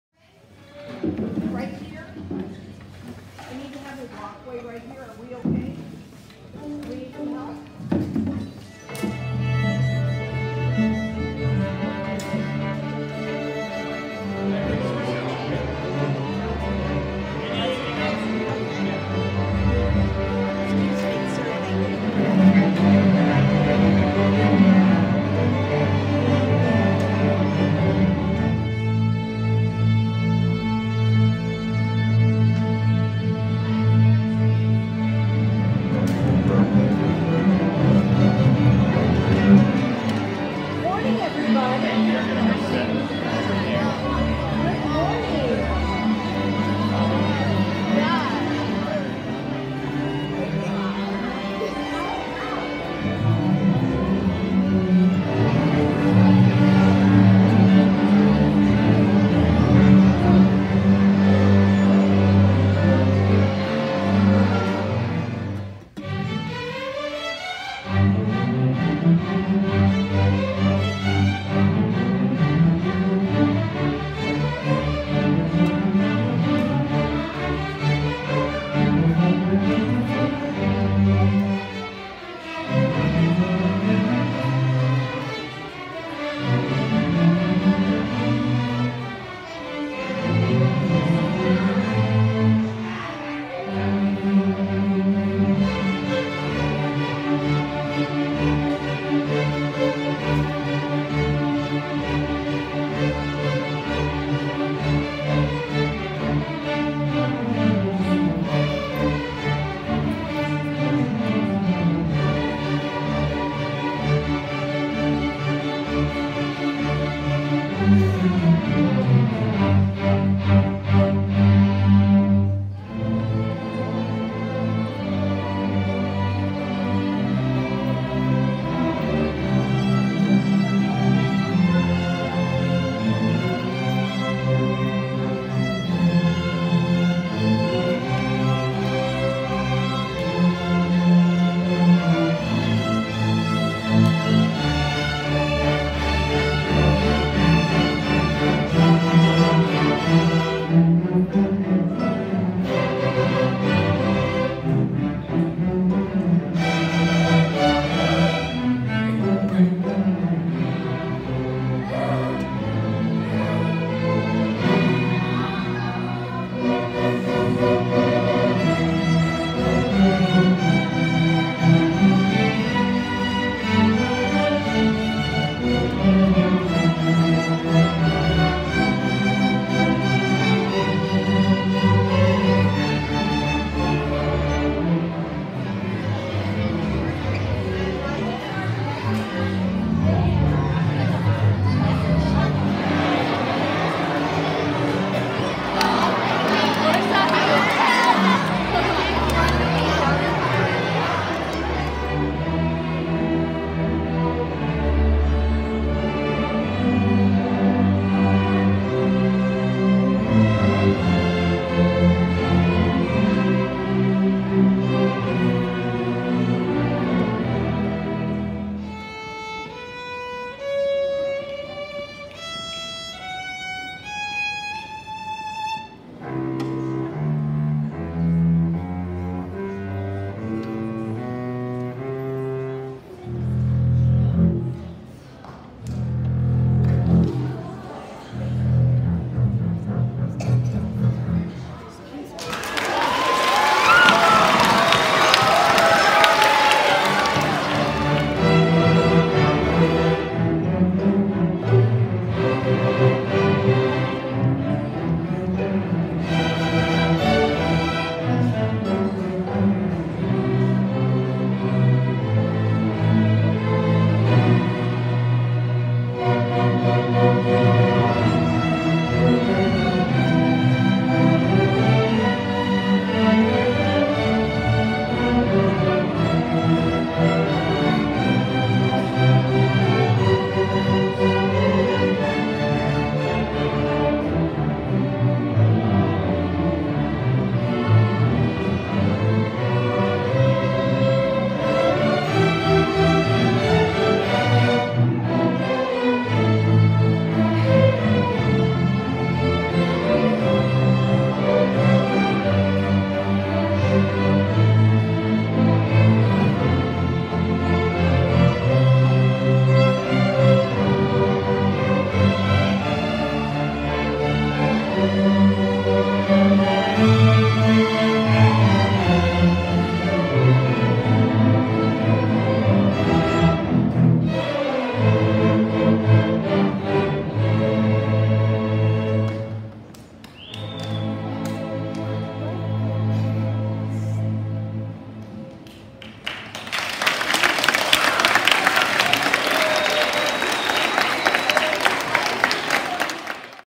2019 AHS Orchestra Tour - Version 2
Director of Orchestras Lorri Wittenkeller leads the Adrian High School Orchestra through the 2019 Elementary School Tour.